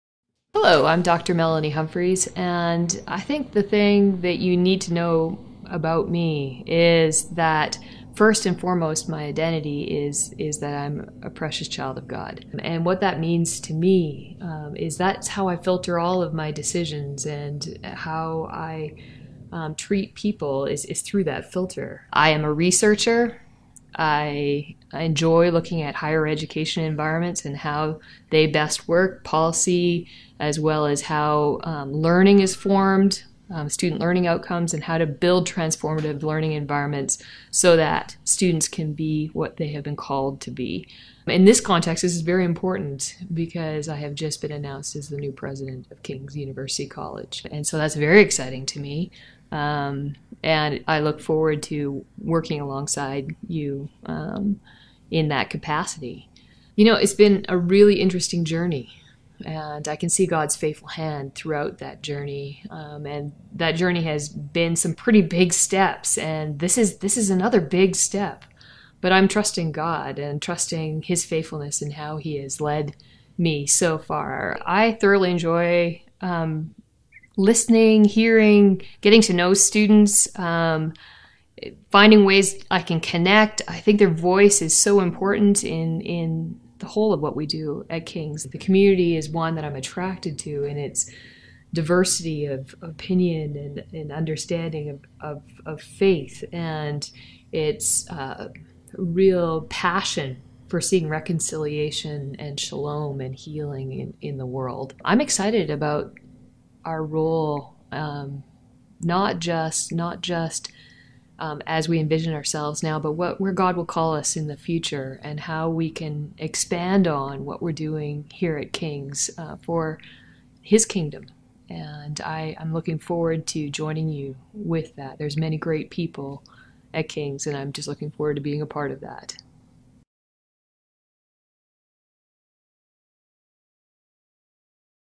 Meet Dr. Melanie Humphreys
Meet Dr. Melanie Humphreys, a gifted and strategic leader, who has been named the fourth president of The King's University College in Edmonton, Alberta effective July 1st, 2013.